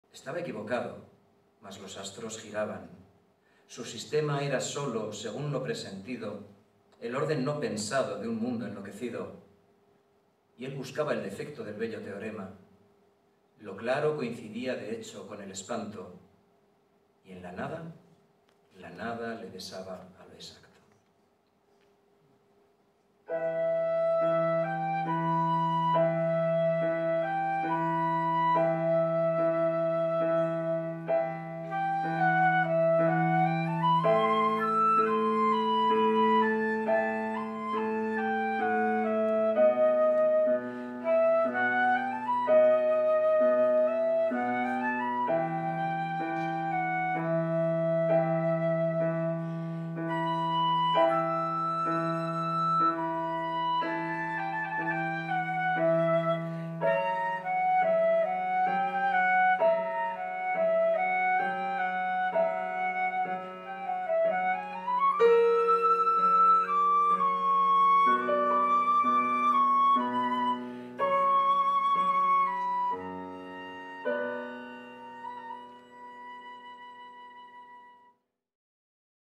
Astros, notas y versos / Carlos Briones, Miriam Ridruejo y Lola Barroso
Espectáculo de Ciencia, Música y Letras de Carlos Briones, Miriam Ridruejo y Lola Barroso en el Festival de Arte y Ciencia 2022.

Organizado por la Cátedra LABORAL Kutxa de Divulgación del Conocimiento y Cultura Científica de la Universidad Pública de Navarra.